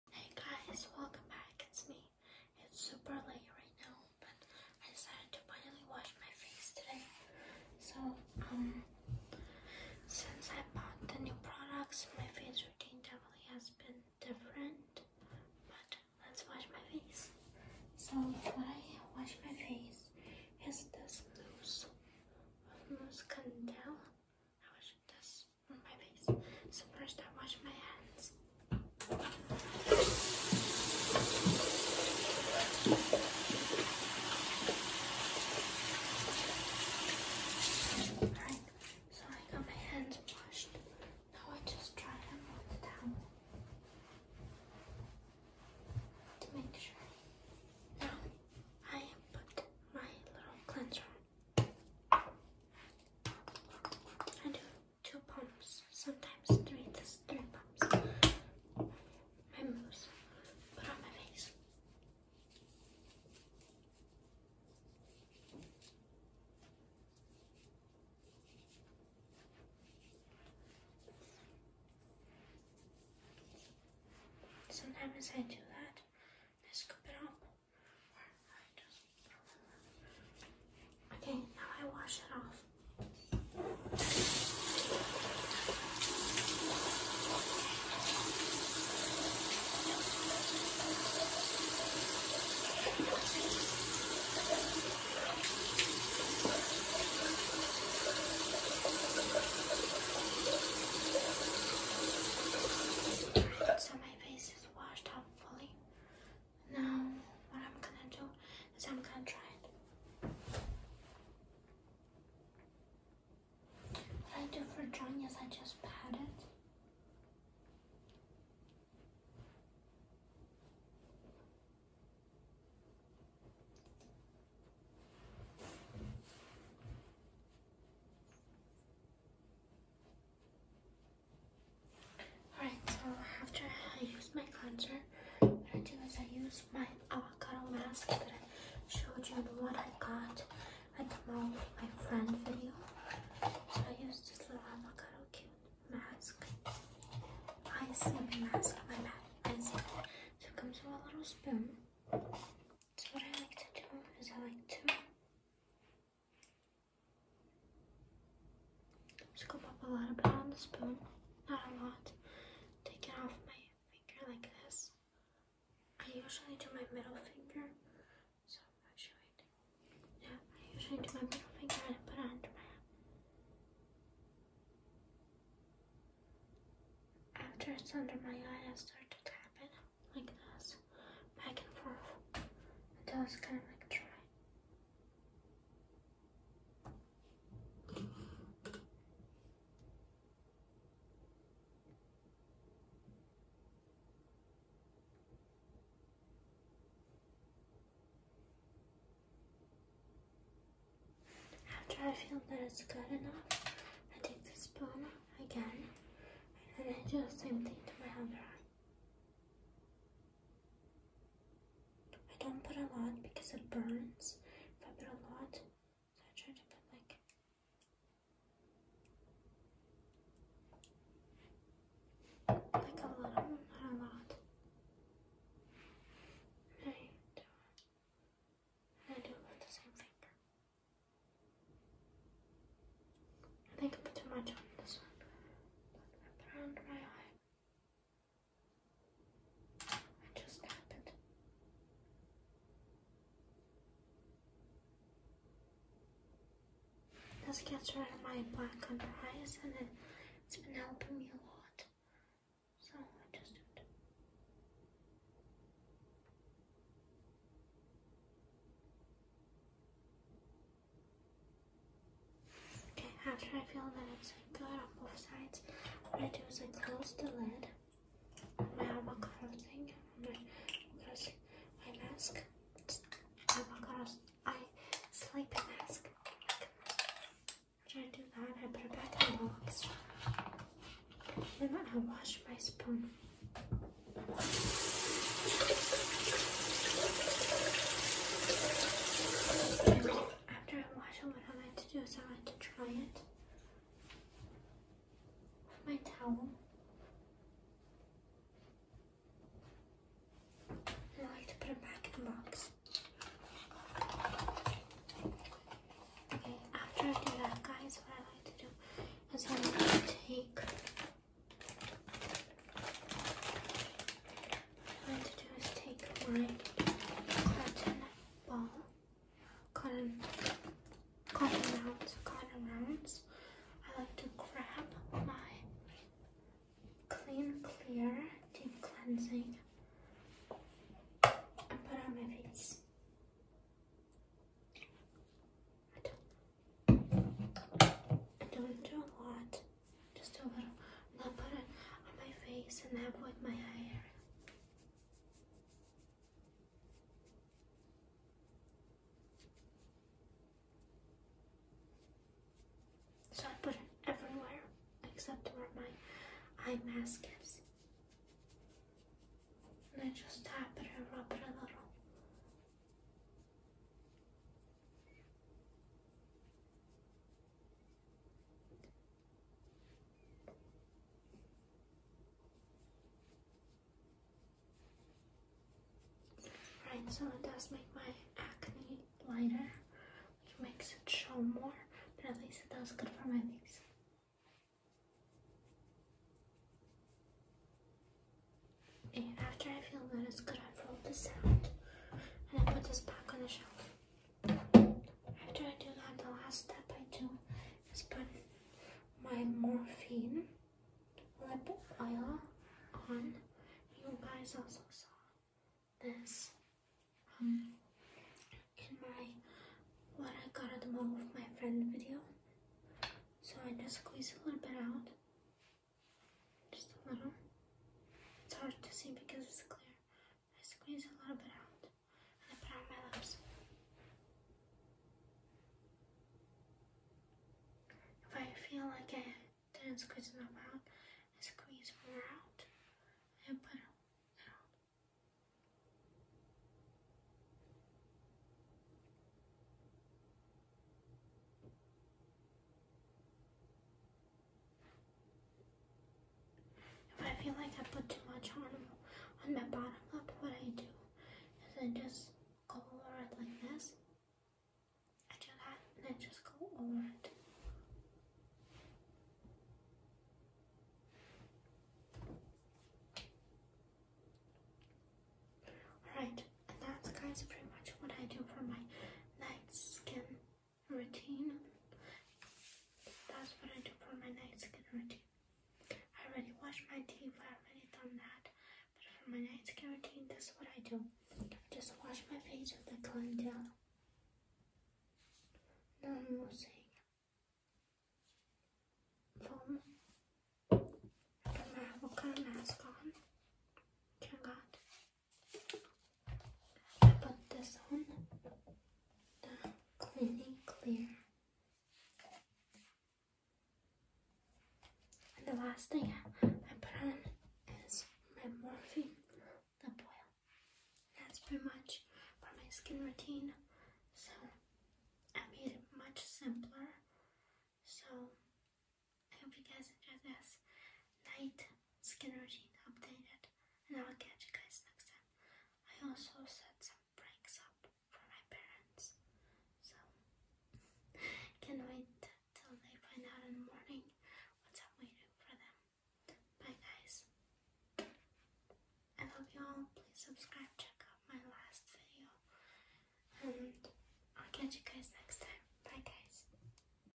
Night skin care routine update
Hey guys welcome back sorry this video is also light I just been going through a hard time in my life this past couple of days but I hope you guy like subscribe and enjoy the video if you guys want to know what happened just tell me in the comments enjoy